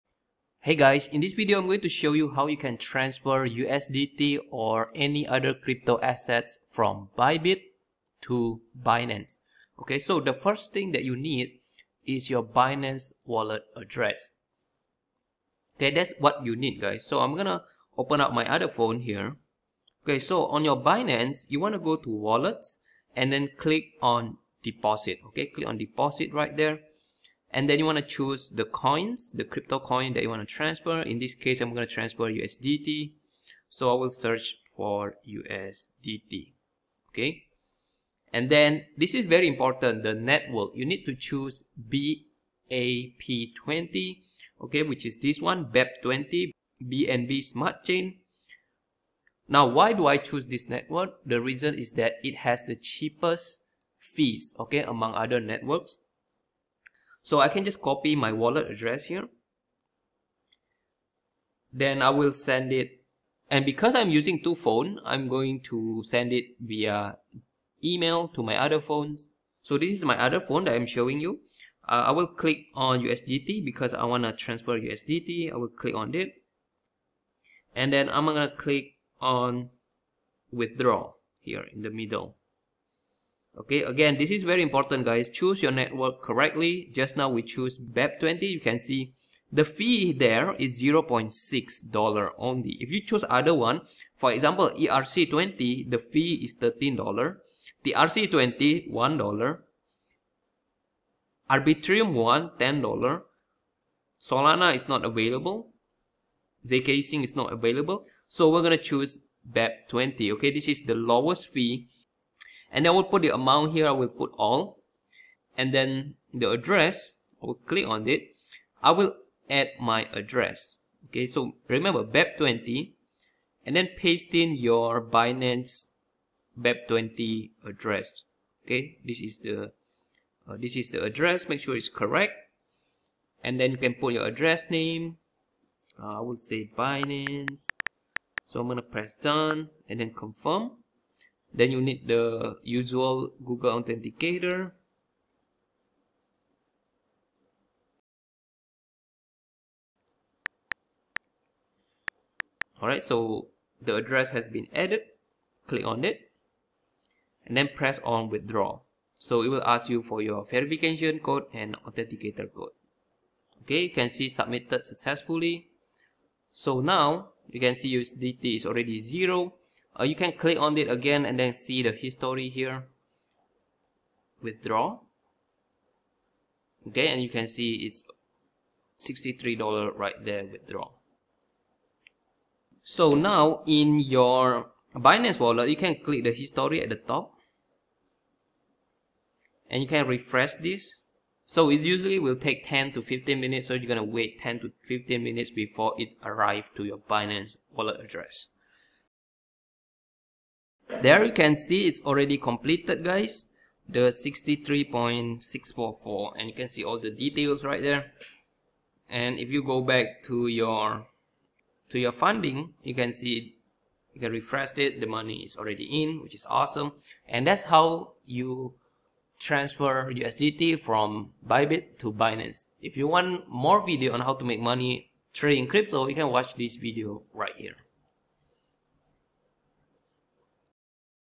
How to Send USDT from Bybit to Binance
Here is how to transfer USDT or any crypto assets from Bybit to Binance wallet. I show you the step-by-step process to transfer USDT USDC on Mobile Phone Android and Apple iPhone ios.

To claim your bonus, you must have purchased AntBot energy.